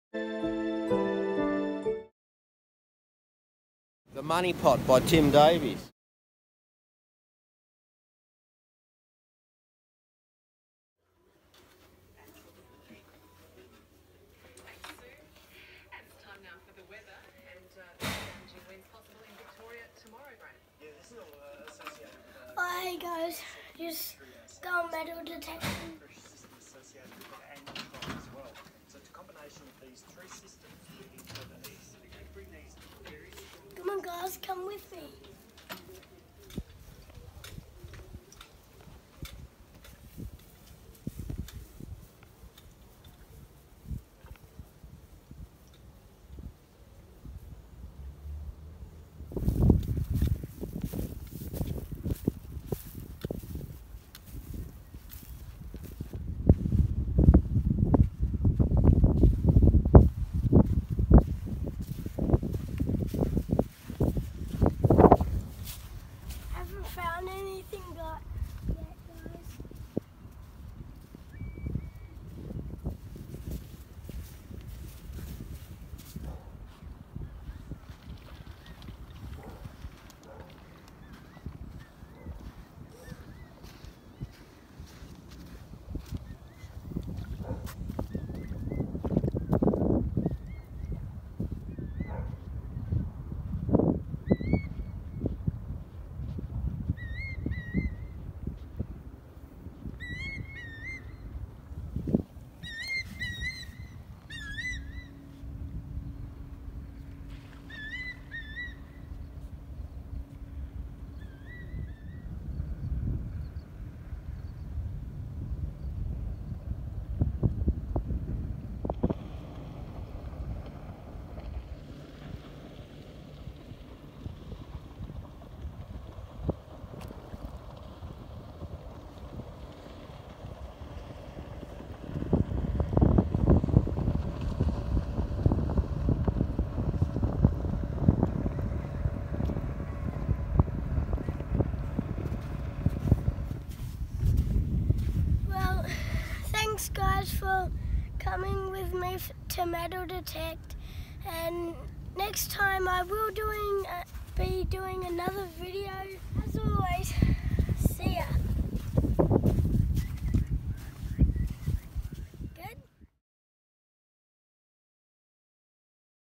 Beach Detecting with Sunny Davies and the Bounty Hunter Tx4 on the Central Coast NSW
Sunny takes the new Bounty Hunter Tx4 out for a test run 🏃
It is really a turn on and go machine very light and even a child can use it easily
Nothing to be found but that’s Treasure Hunting